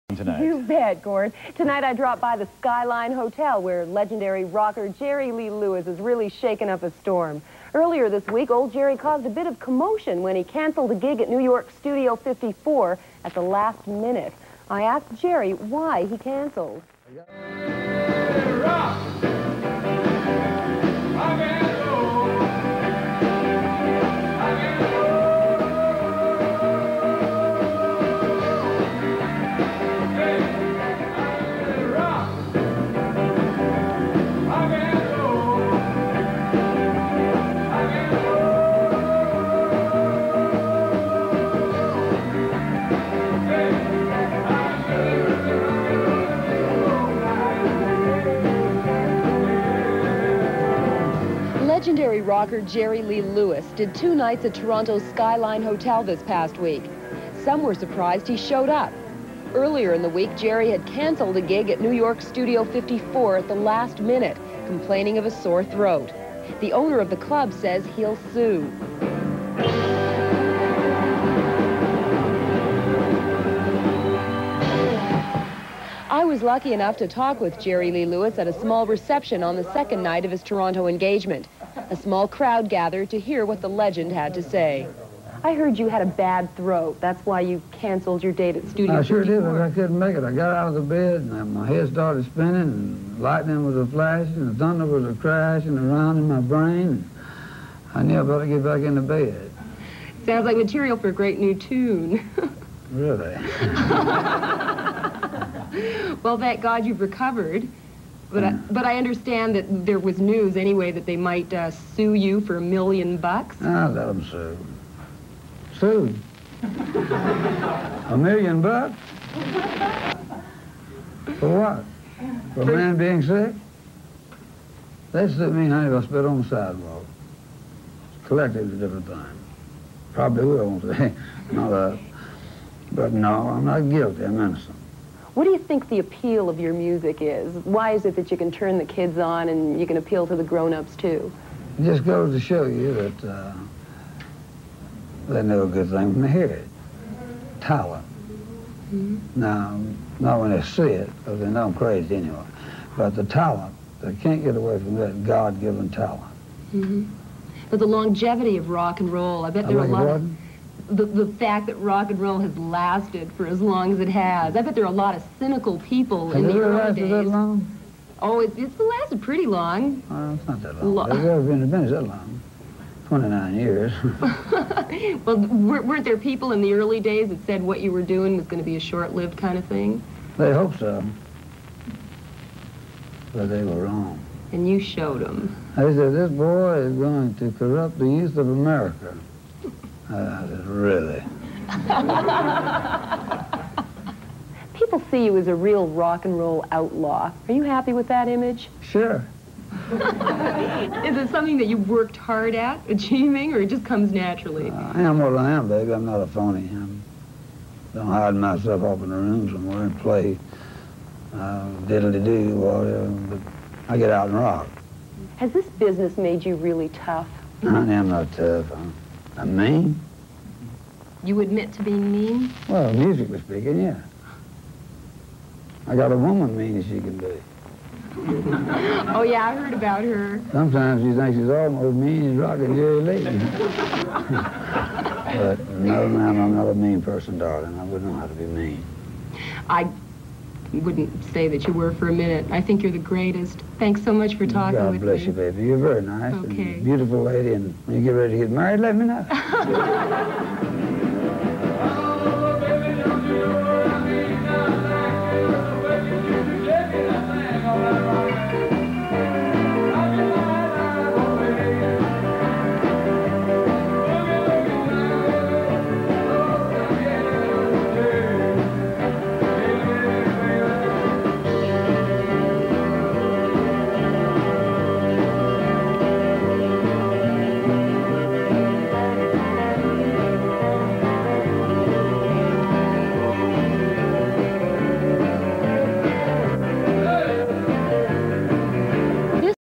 Studio 54 Sues Jerry Lee Lewis
Jerry Lee Lewis Cancels Studio 54 - Gets Sued 1984 interview at Toronto Skyline Hotel

the time Jerry Lee Lewis f’d over Steve Rubell at Studio 54